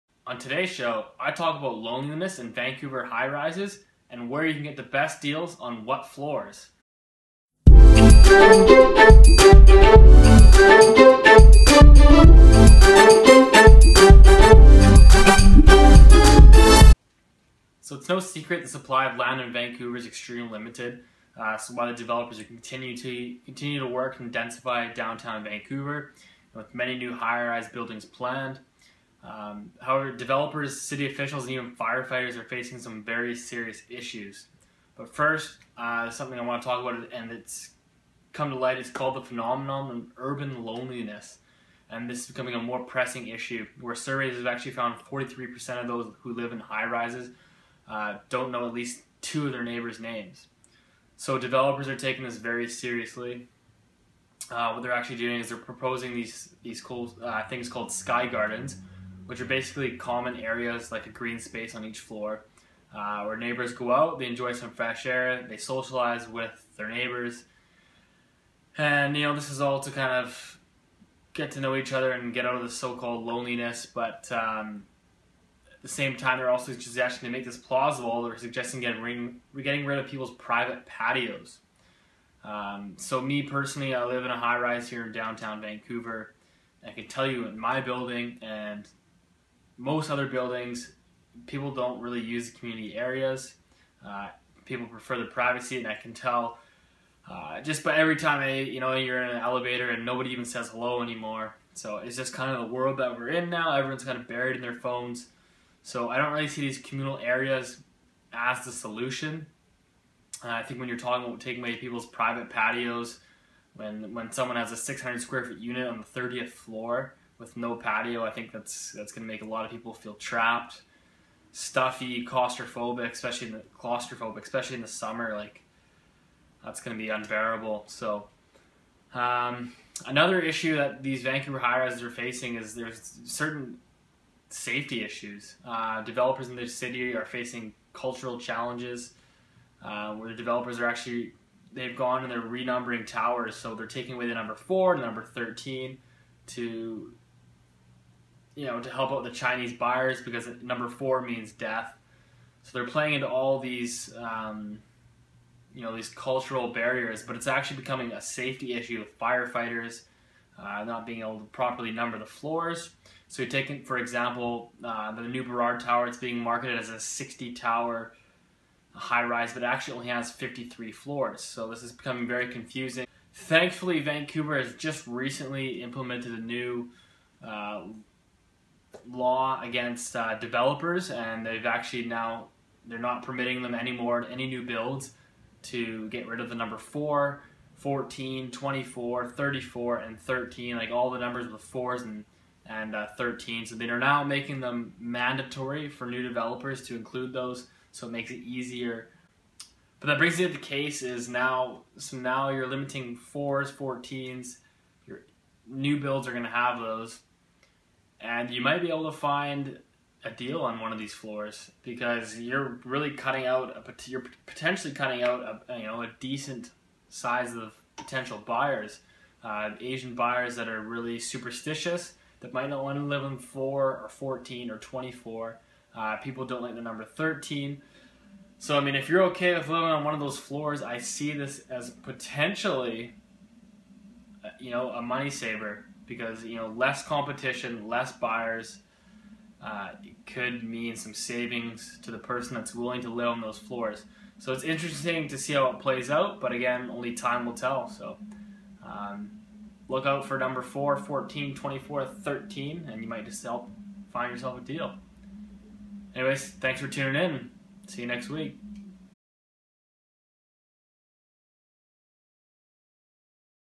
Vancouver Realtor: Loneliness & Scoring Deals on floor #4
In this video Vancouver Realtor Steve Saretsky talks about recent loneliness issues regarding Vancouver high rises and how you might be able to score a deal on floor #4.